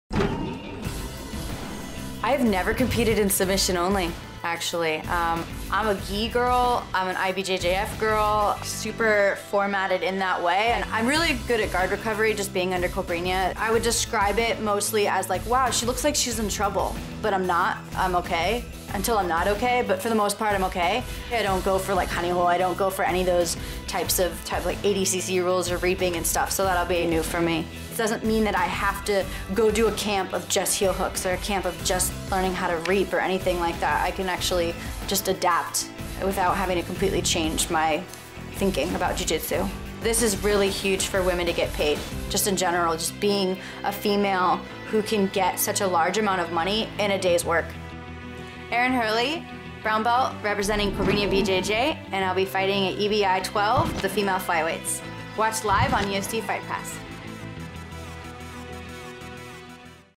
EBI 12 The Female Flyweights: Erin Herle
@erinherle representing Cobrinha BJJ makes her debut on the Submission Only stage in search of becoming the first EBI Womans Flyweight Champion on Sunday July 30th at EBI 12 "The Female Flyweights" live from The Orpheum Theatre in Downtown L.A tickets on sale now or watch live on UFC Fight Pass